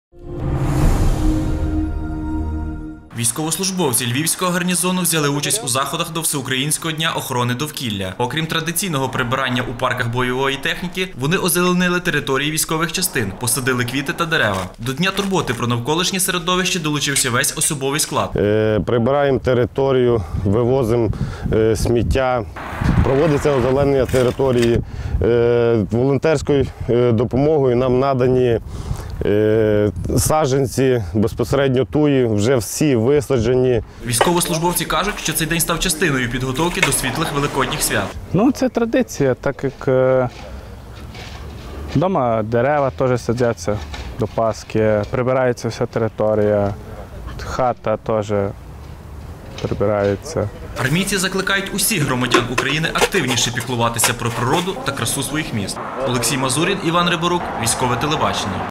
День довкілля на Львівщині
Військовослужбовці Львівського гарнізону приєдналися до всеукраїнського дня охорони довкілля. Окрім традиційного прибирання у парках бойової техніки, вони озеленили територію військових частин  - посадили квіти та молоді дерева.